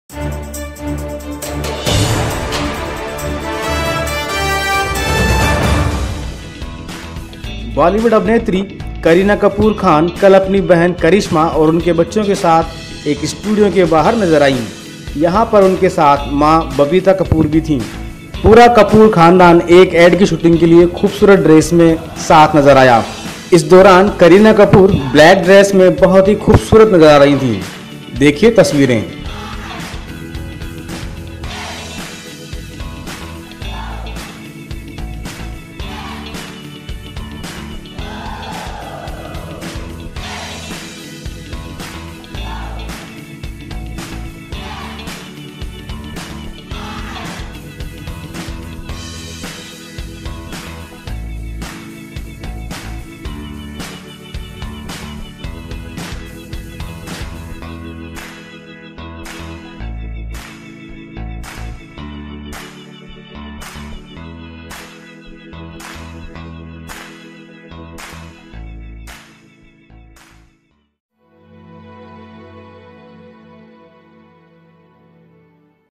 करीना कपूर और करिश्मा कपूर एक विज्ञापन शूट में साथ आई नज़र, देखें तस्वीरें
बॉलीवुड अभिनेत्री करीना कपूर खान कल अपनी बहन करिश्मा और उनके बच्चों के साथ एक स्टुडियों के बाहर नज़र आईं. यहां पर उनके साथ मां बबीता कपूर भी थी. ये पूरा कपूर परिवार एक एड की शूटिंग के लिए खूबसूरत ड्रेस में साथ नज़र आया. इस दौरान करीना कपूर ब्लैक ड्रेस में बहुत ही खूबसूरत नज़र आ रही थीं.